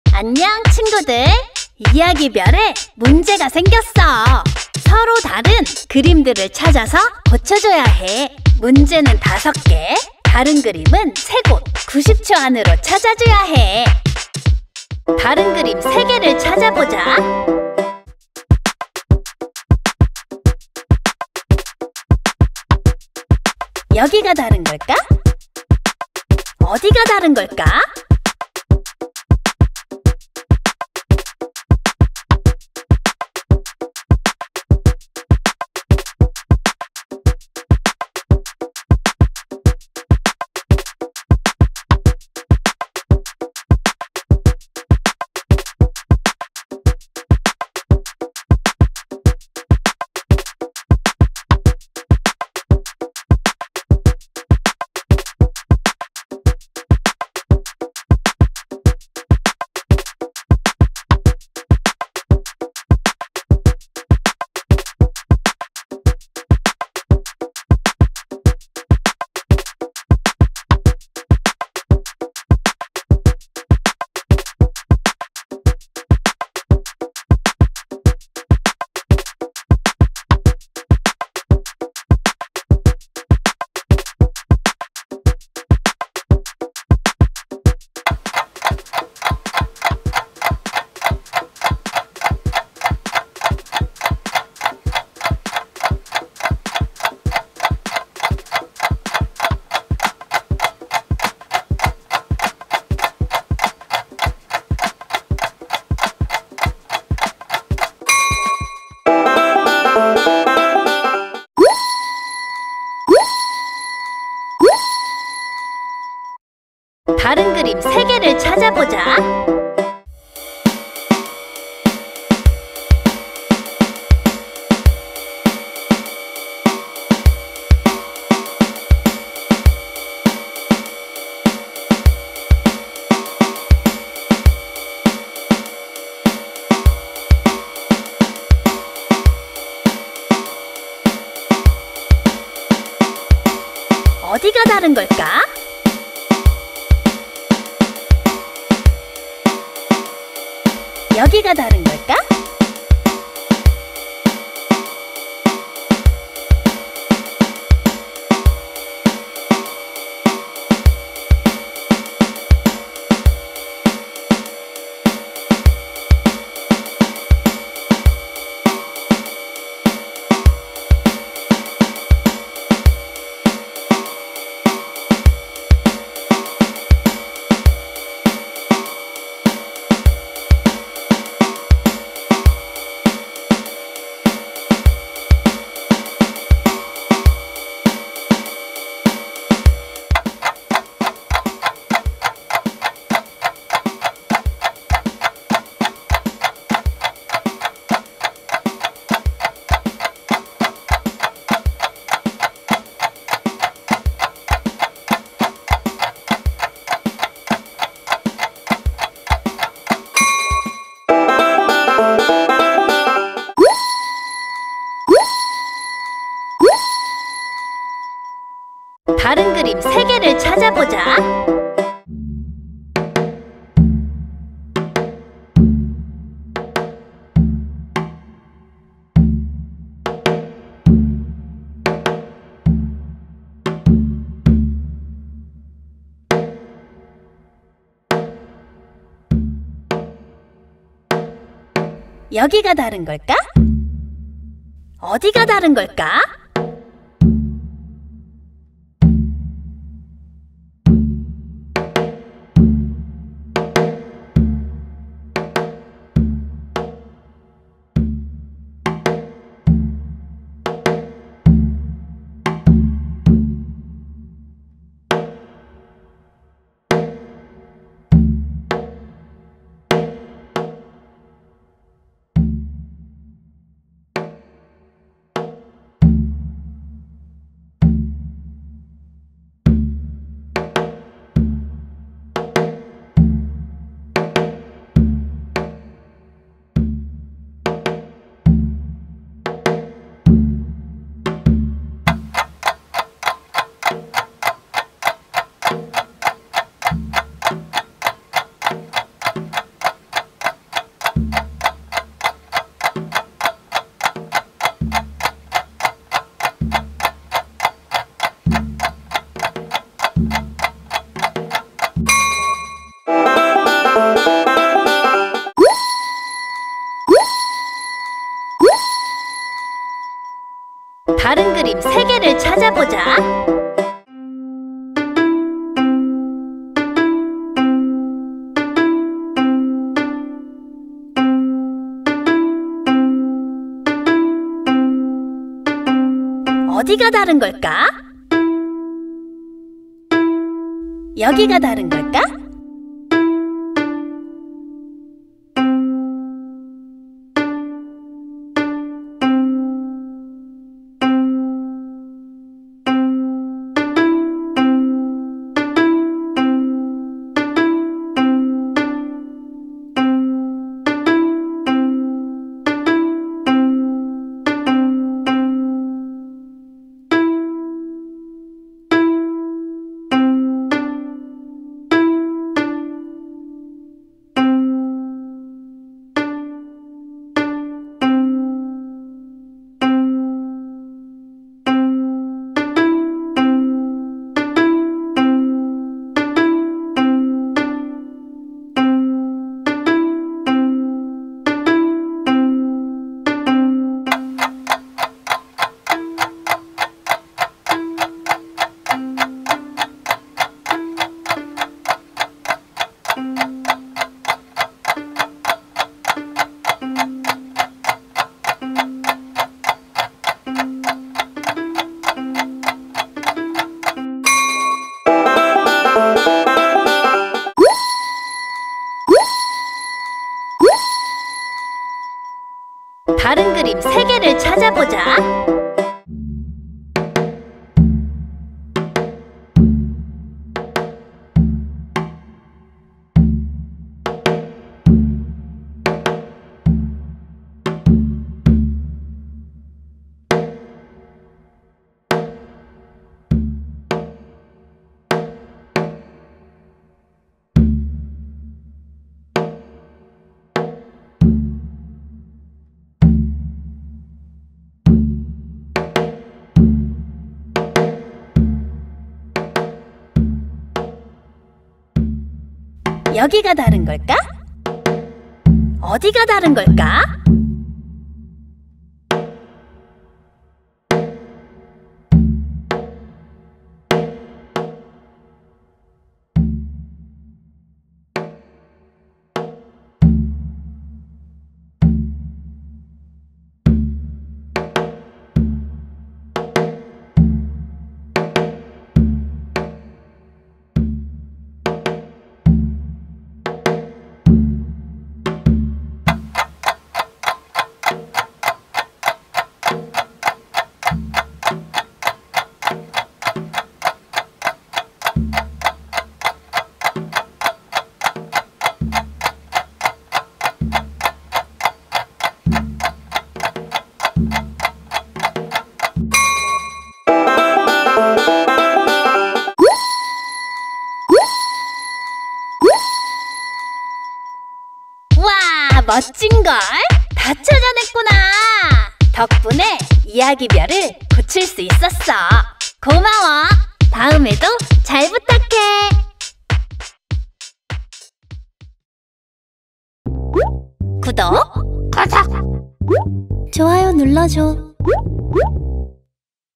[채리의 다른 그림 찾기🍒] 틀린 그림 찾기 | 견우와 직녀 별 이야기 | 체리키즈
채리🍒꾸미🦊쥬빌레🐥와 함께하는 다른 그림 찾기🚀
채리, 꾸미, 쥬빌레와 이야기 행성을 원래대로 만들어주자~

안녕 친구들~
구미호 별 이야기에 문제가 생겼어!
서로 다른 그림들을 찾아서 고쳐줘야 해.
다른 그림을 찾아서 별 이야기를 다시 원래대로 만들자🚀